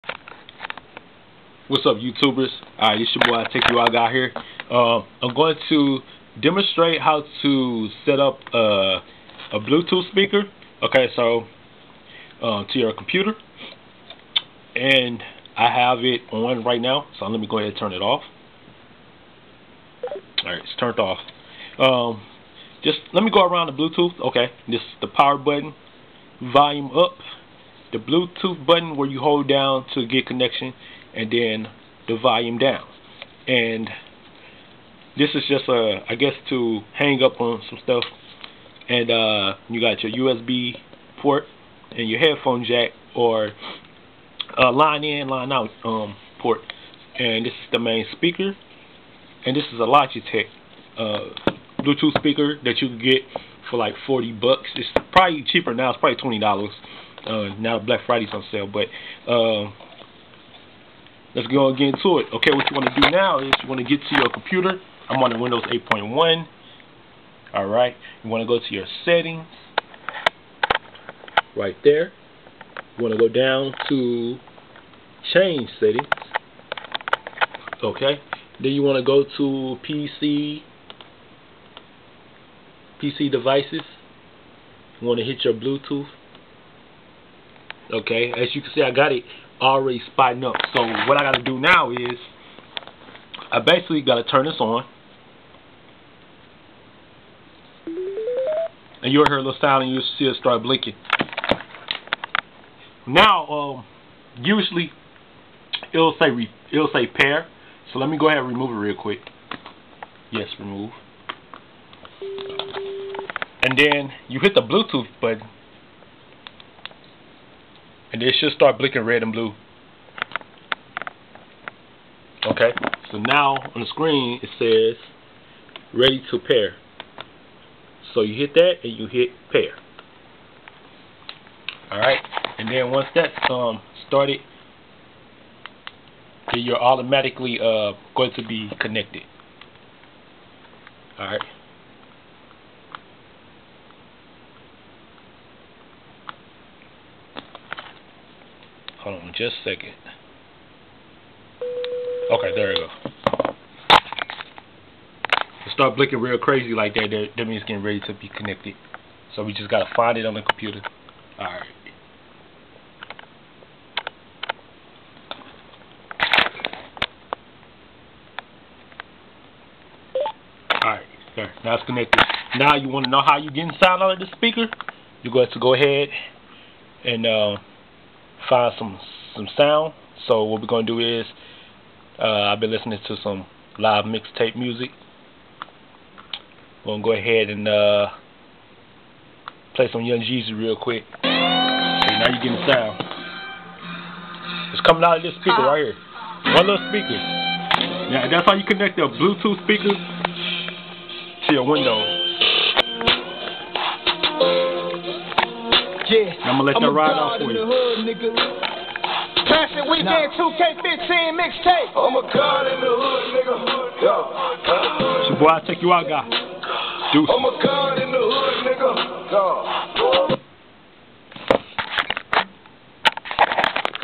How To Connect Bluetooth Speakers To Windows 8.0/8.1/10 PC
Hello everyone, here is a great useful tutorial of how to connect a bluetooth speaker to your PC.  This is a Logetic Bluetooth speaker. It cost me about $40.  Might be cheaper than that by now.  But it is a great speaker you can use in a small decent room.  If you just needing something for media consuming then this is a good one.